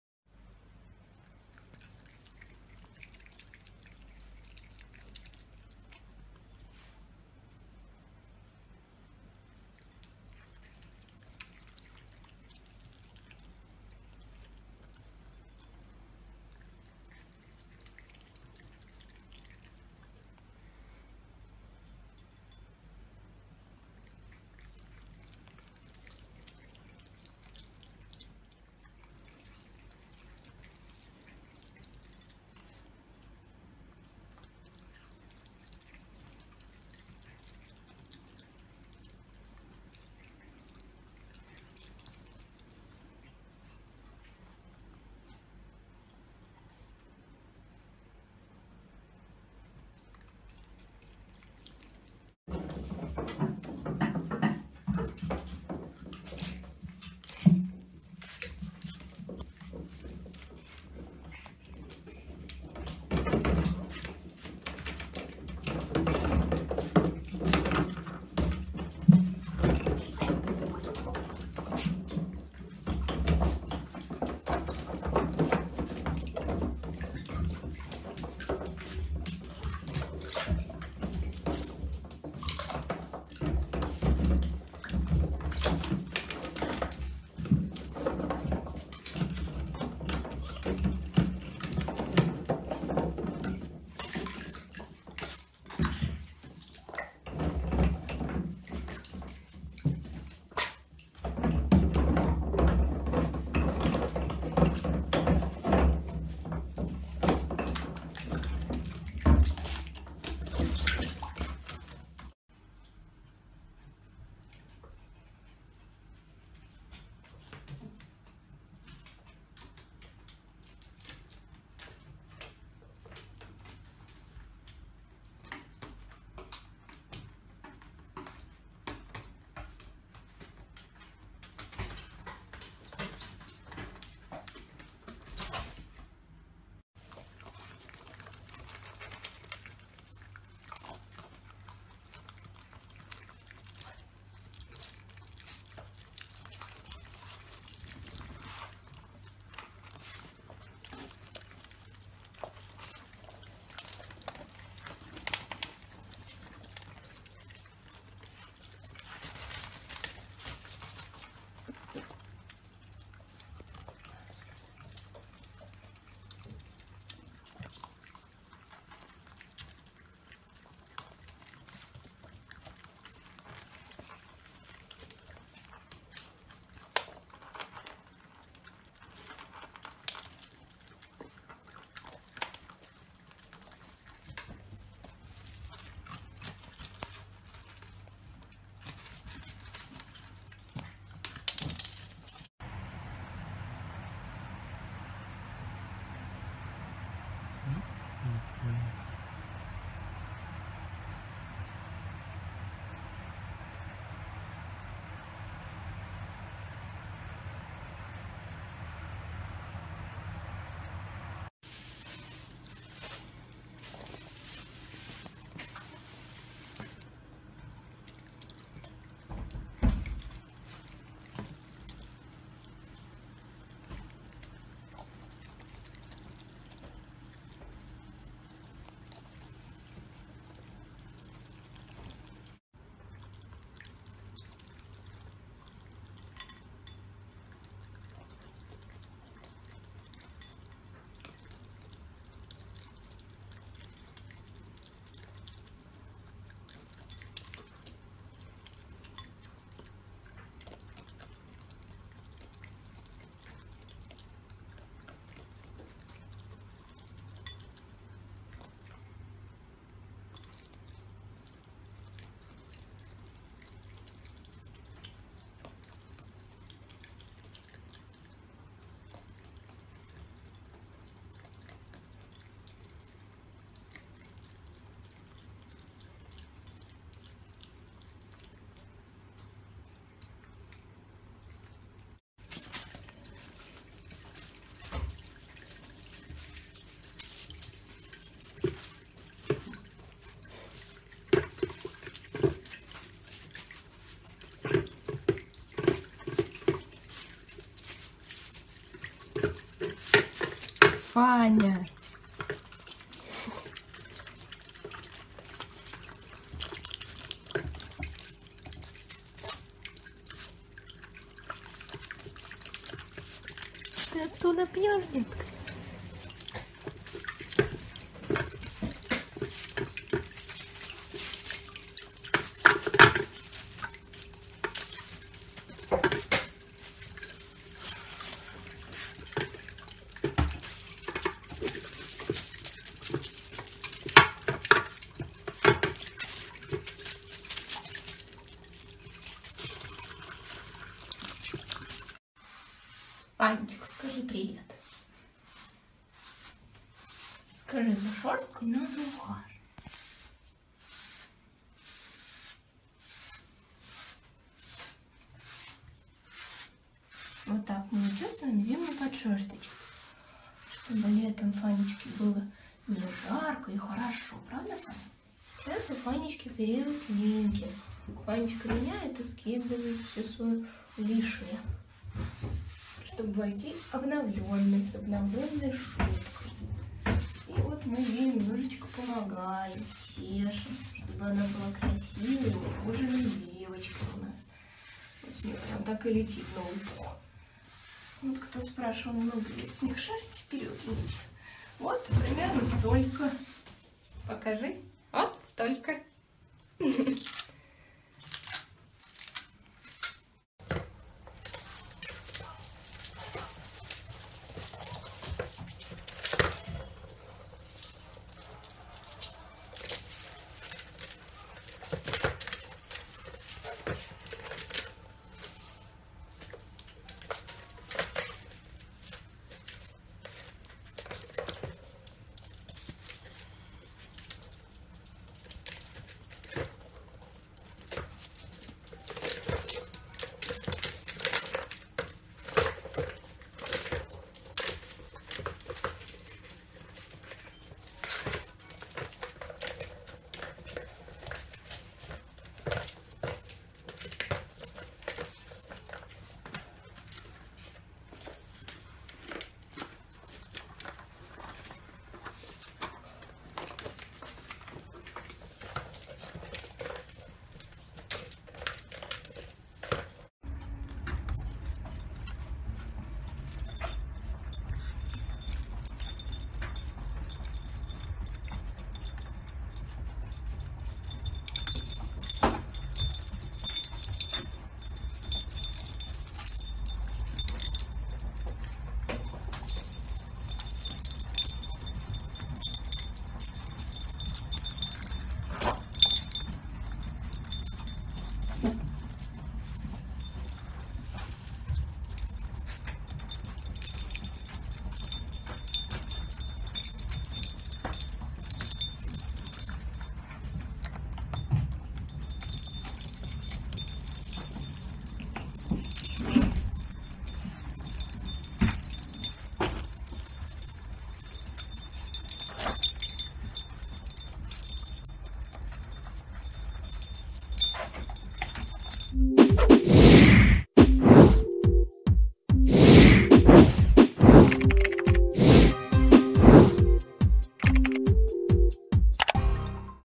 Raccoons Wearing Clothes - FUNNIEST Compilation (2021)
Raccoons are adorable, fluffy, and super cute. Although they seem a bit scary, they're quite friendly when treated well like most animals. This is simply the best, funniest, and cutest compilation of raccoons on youtube. Enjoy!